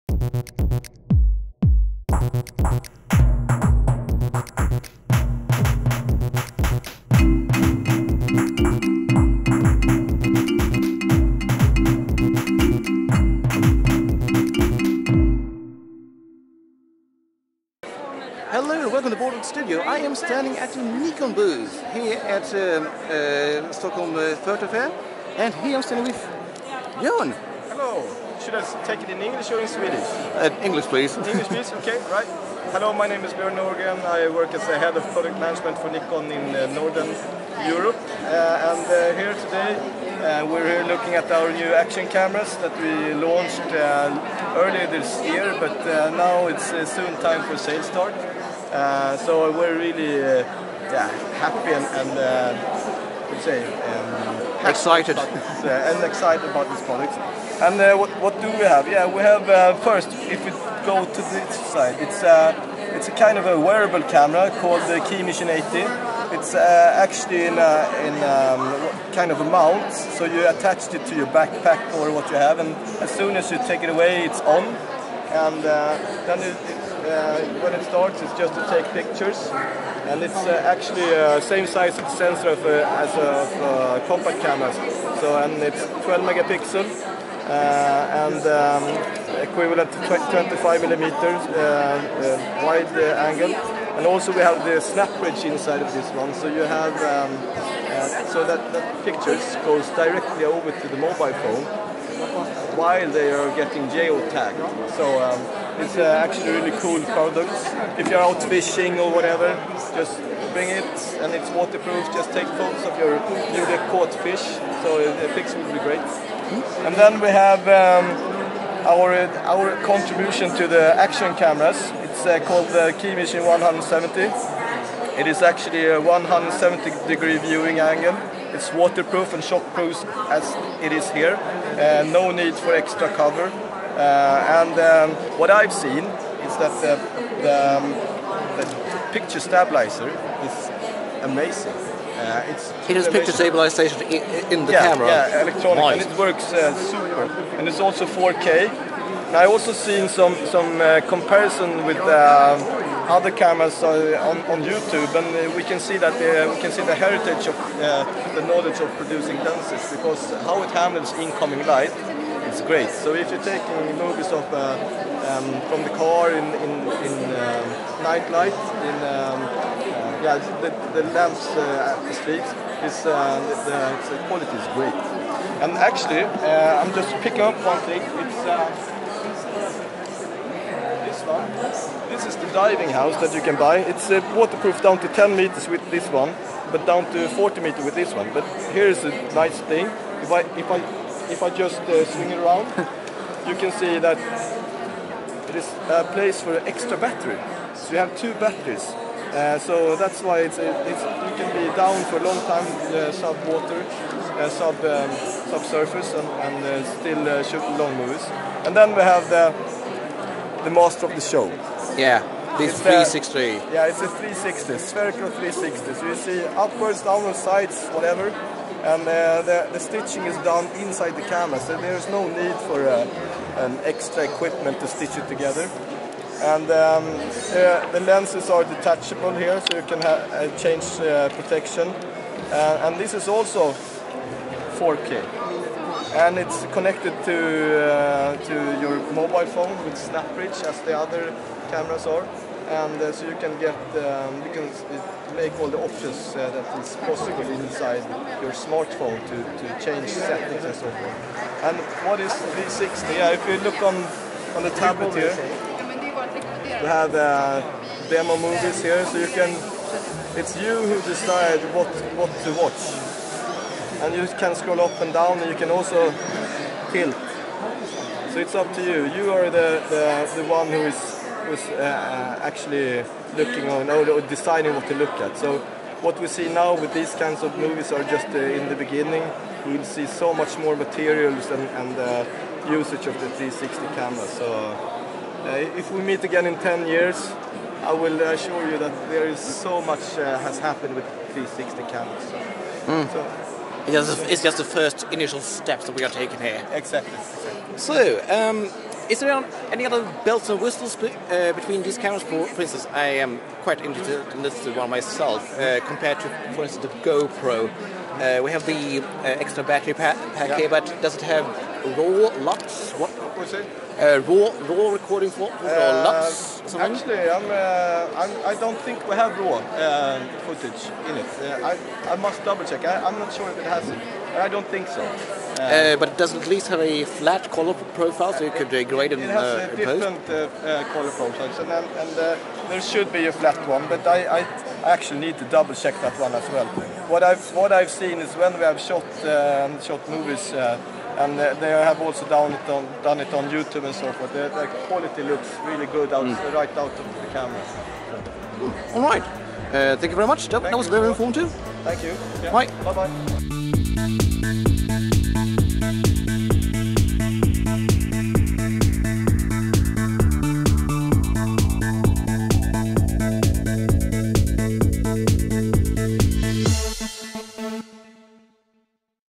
Stockholm Photo Fair 2016: Nikon Action Cameras
I’m at Stockholm’s photo fair interviewing the sellers about their new gear. In this Video im looking at Nikons new action cameras.